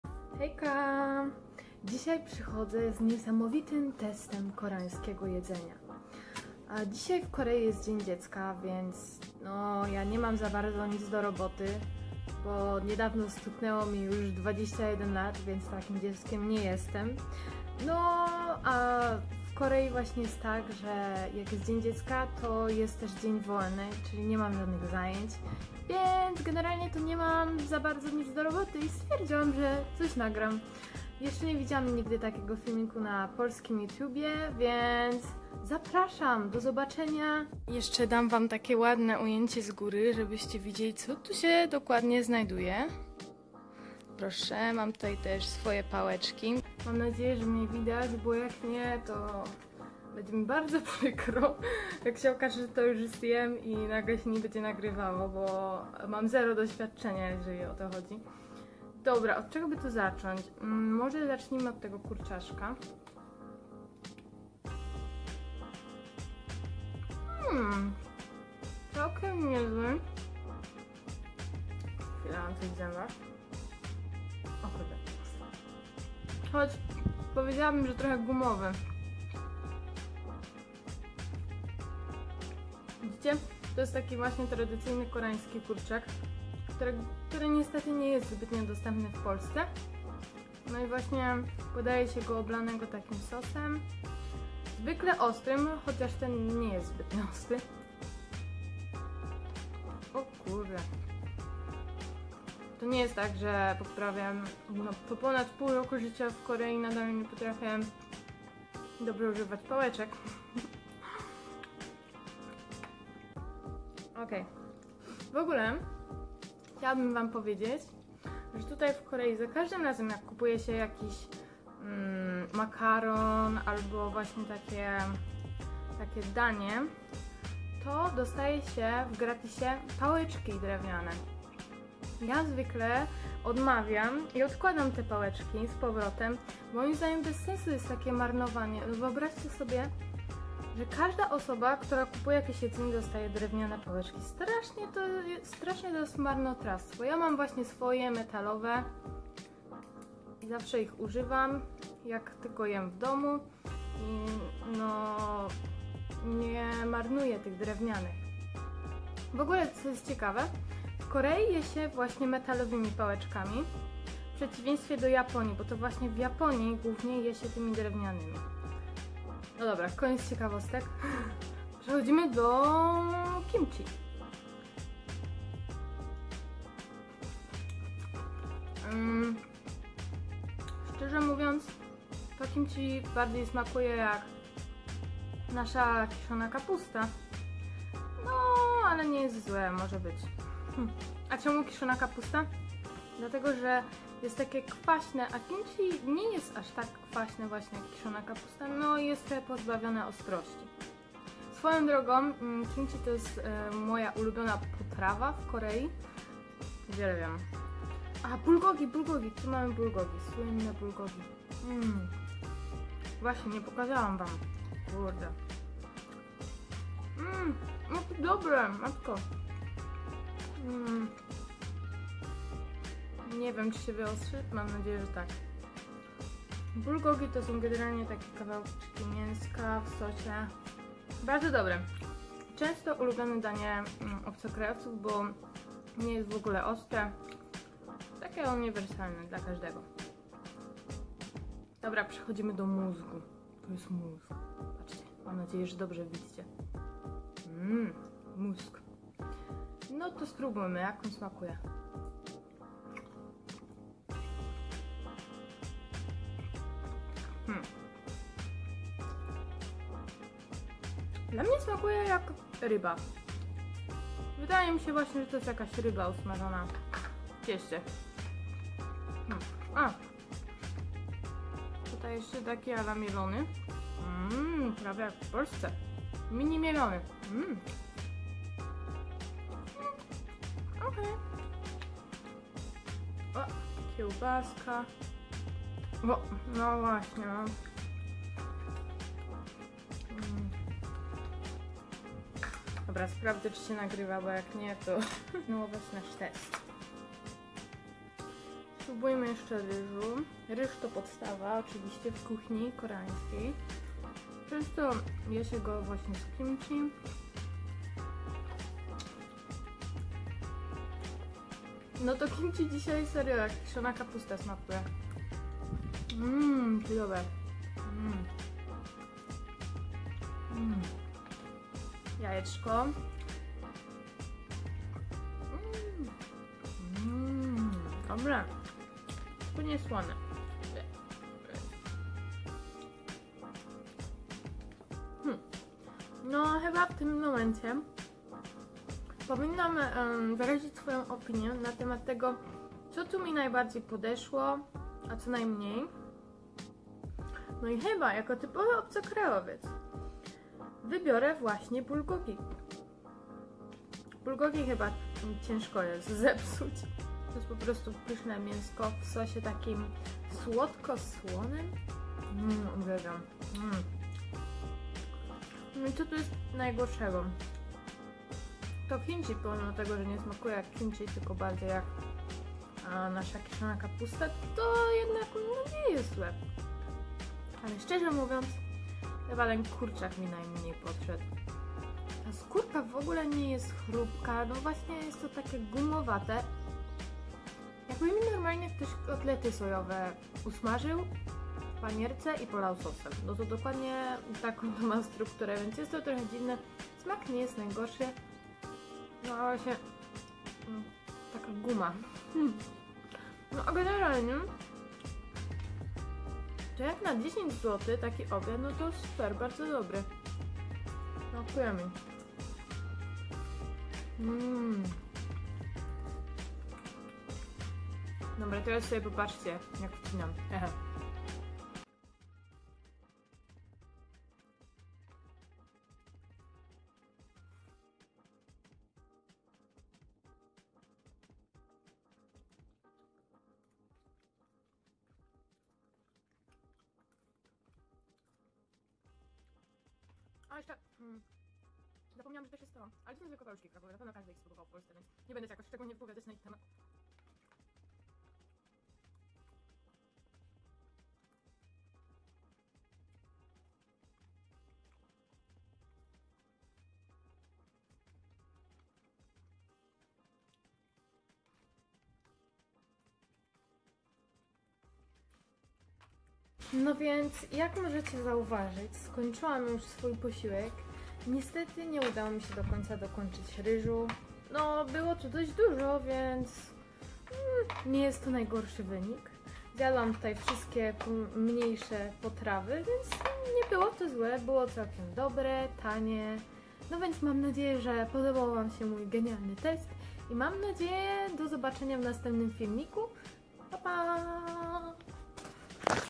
JEM MÓZG?! 🤣 - MUKBANG Z KOREAŃSKIEGO SKLEPU
Hejka!
To mój pierwszy film, mam nadzieję, że się spodoba :D.

Nazywam się Basia i od września jestem na wymianie w Korei Południowej. Uwielbiam kuchnię koreańską, chodzić po górach oraz uczyć się koreańskiego.
Mój instagram: basior181